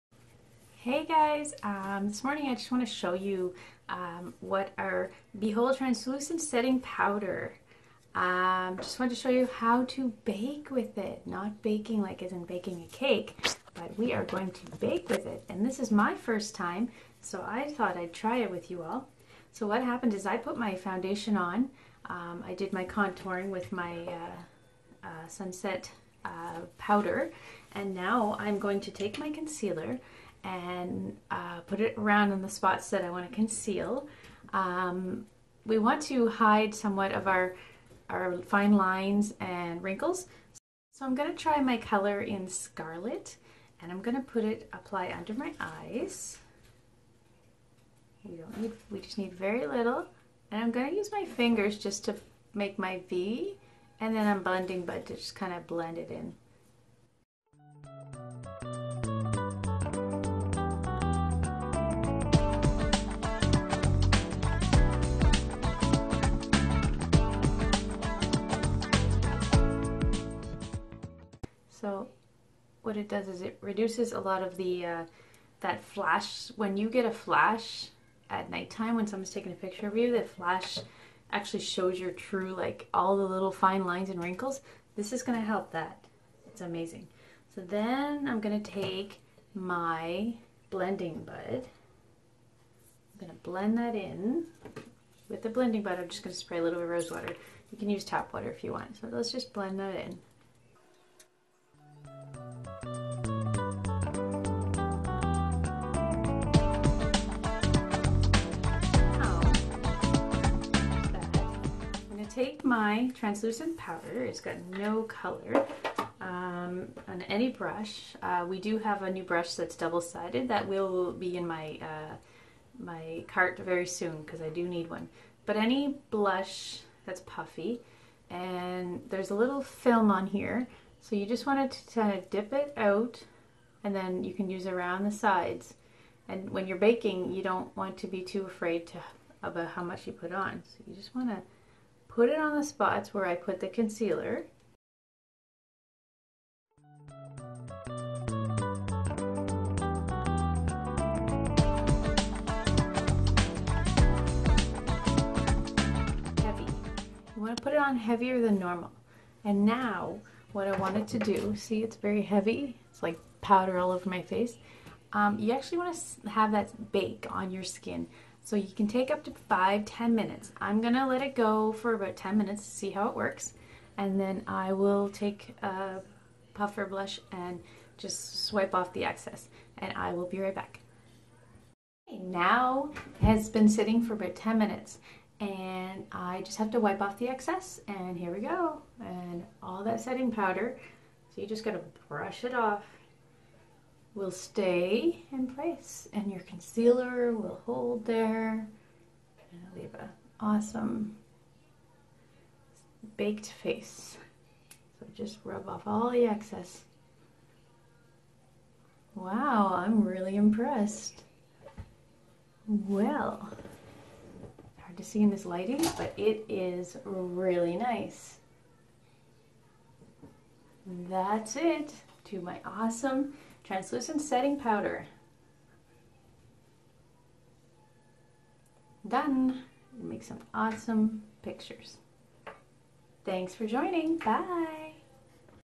Bake with Youniques Setting Powder
Let me show you how I bake with Younique’s Translucent Setting Powder and concealer.